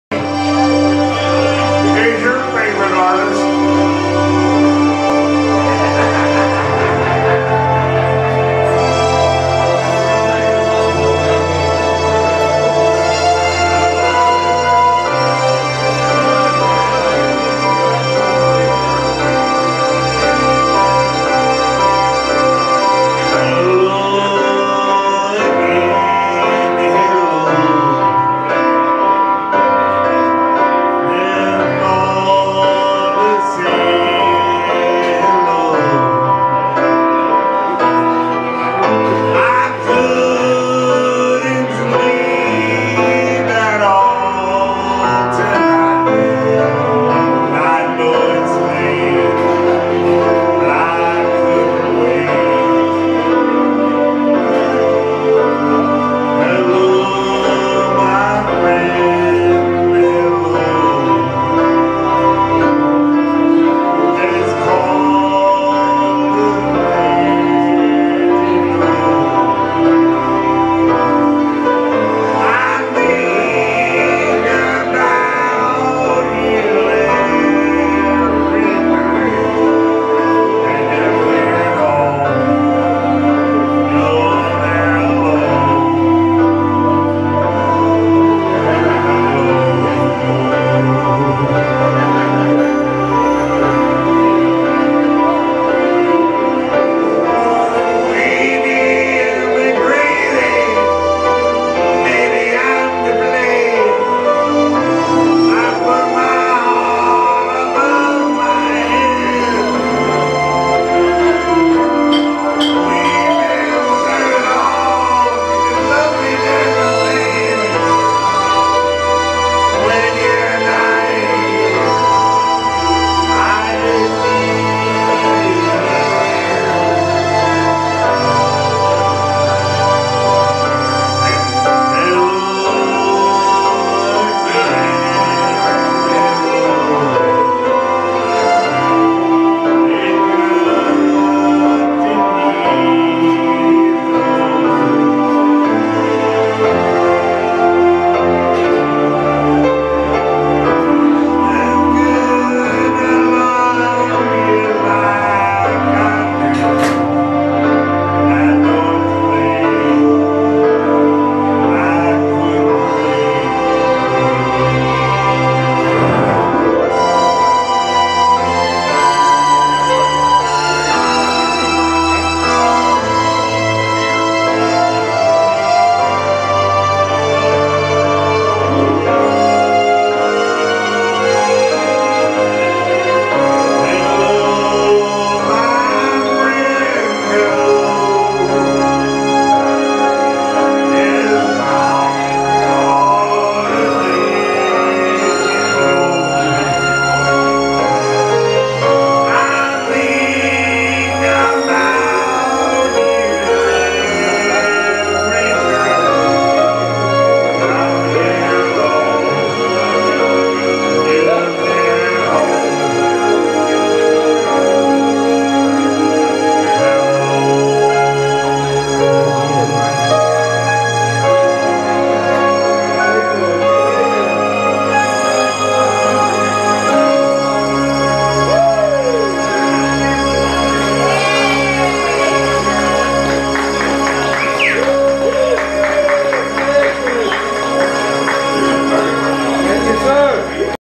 Last Days of the Ash Street Saloon ~ John "Elvis" Schroeder
Recorded LIVE at the Ash Street Saloon Portland Oregon
John "Elvis" Schroeder The "King" of Portland Oregon
sings some of our favorites for christmas and for his very special friend Tracy and the Ash Street Saloon crew ~ Merry Christmas 2017 ~ Hello